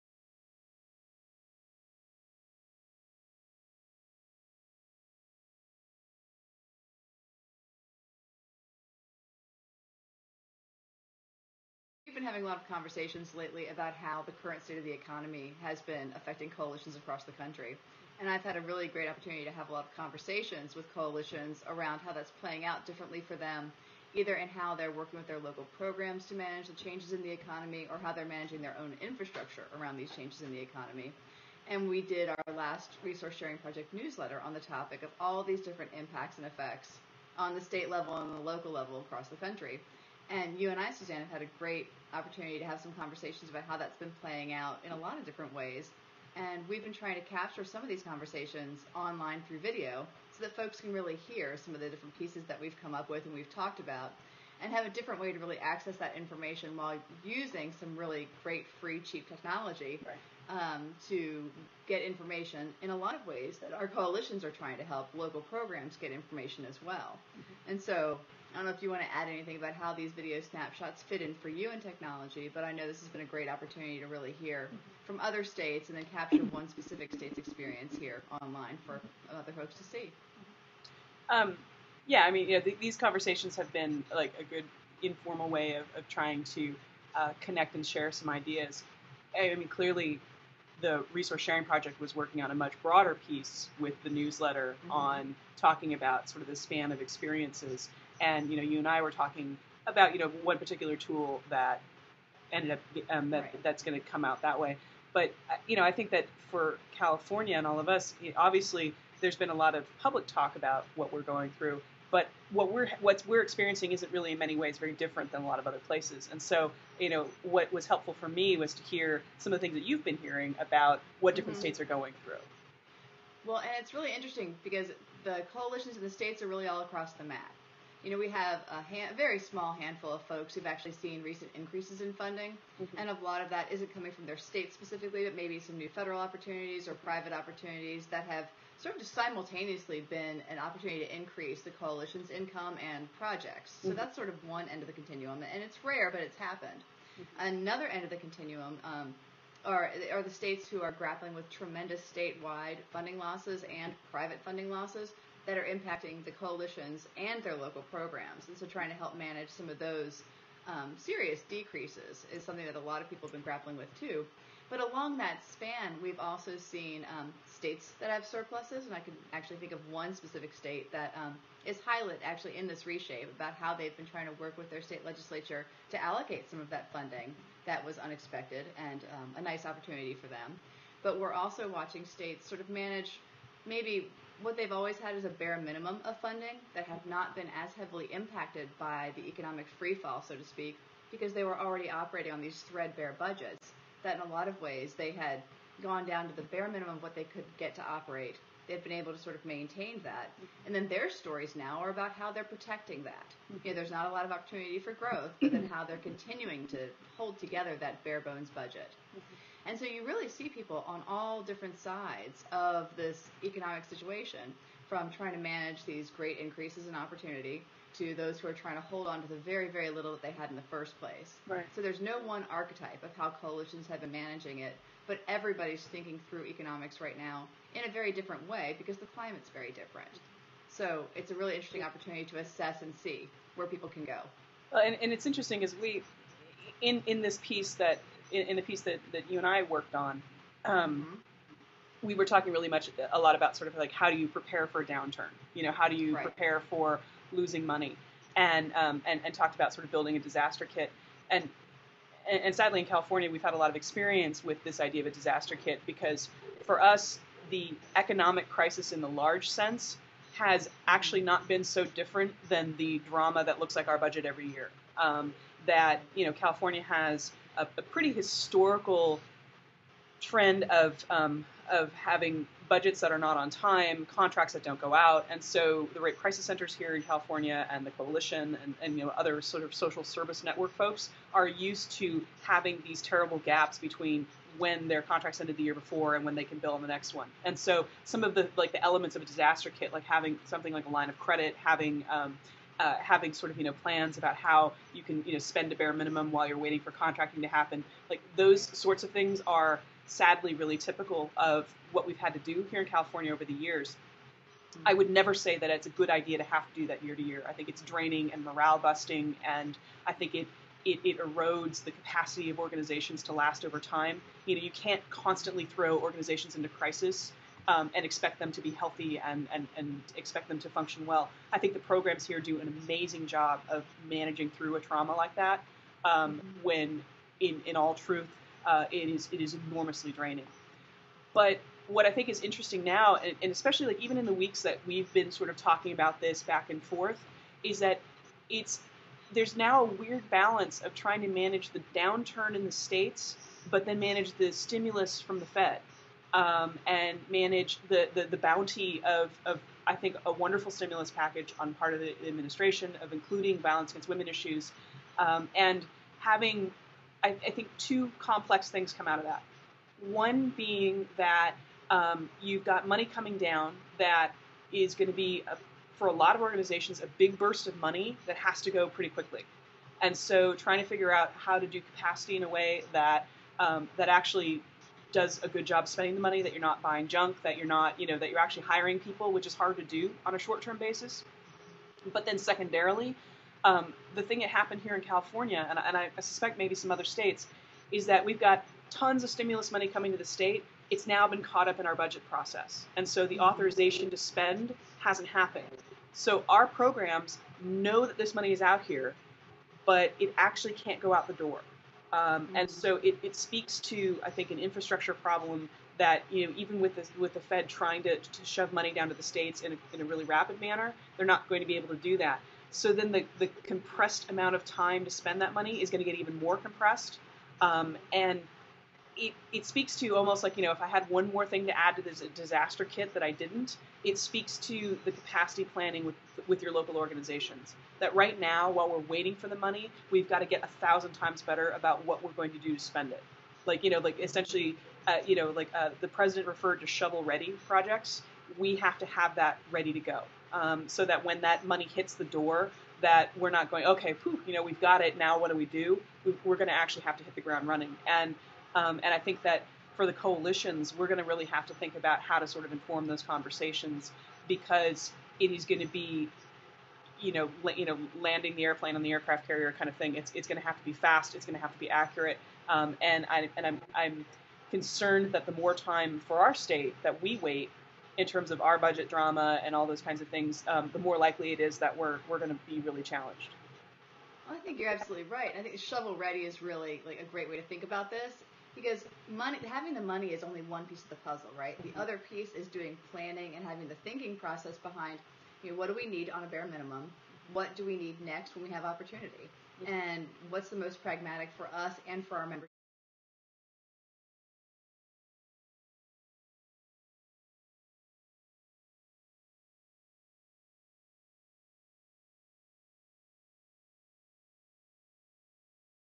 RSP Introductions
This is part of a series of informal conversations about economic crisis and recovery that served as background for an article for the Sexual Assault Coalition Resource Sharing Project.